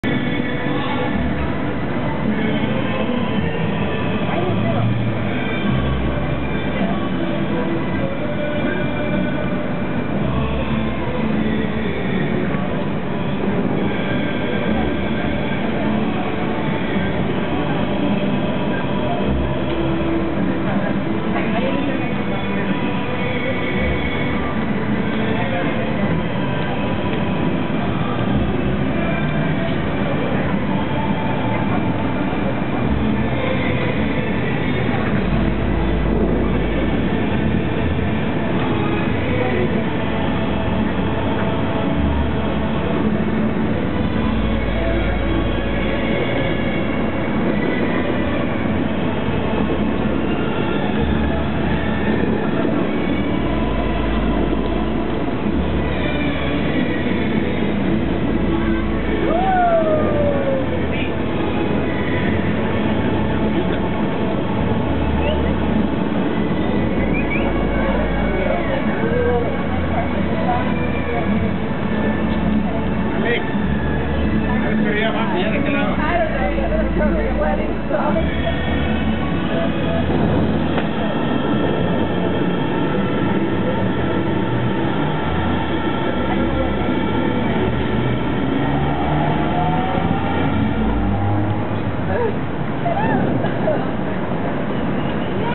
Bellagio Fountains
Choreographed water show at the Bellagio.  Happens every 30 minutes.  This is Weds April 20, 3pm as we were walking away from the hotel/casino complex.  Stunning.